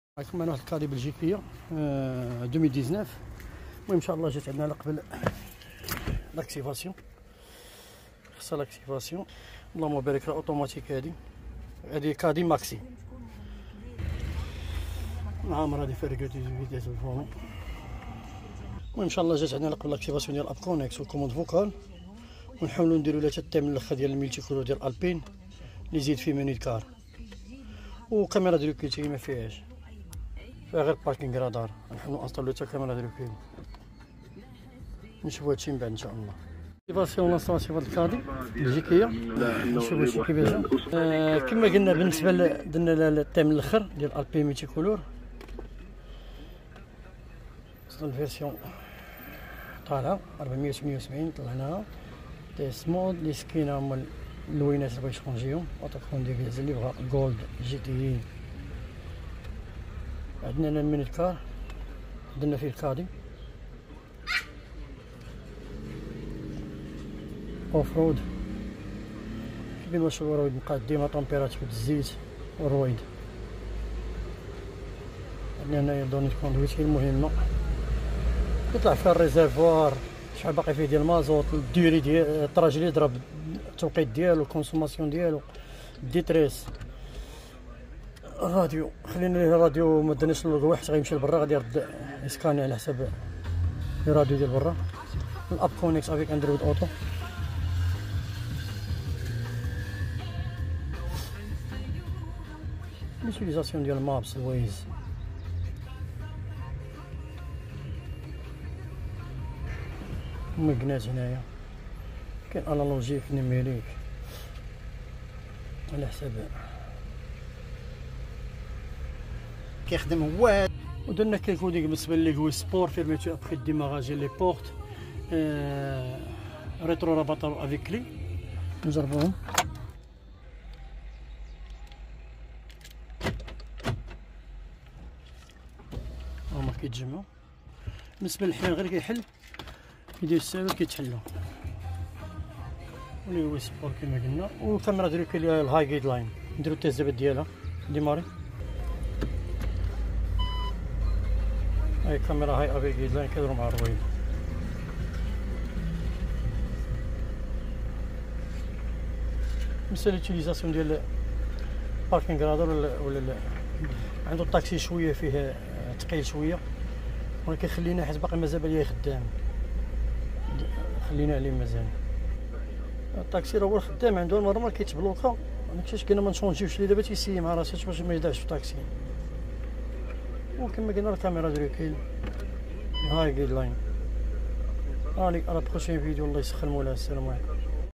VW Caddy 2019 Pack D'activé V4 Carplay AndroidAuto Thème Alpine MenuCar Caméra Guideline☎️0661482268
VW Caddy Fin 2019 De Belgique

🏝 Pack D'activation V4

✅️ App Connect Carplay Android Auto Mirrorlink
✅️ Thème Alpine Multicooleur et Menu Car
✅️ Moniteur Sport & Offroad
✅️ Commande Vocale
✅️ Coding & Logos Stations

🏝 Installation Caméra High Guideline Poingé

Pour VW Golf 7 Tiguan Touran Passat CC Caddy Jetta

Auto-Coding Maroc
Salmiya Casablanca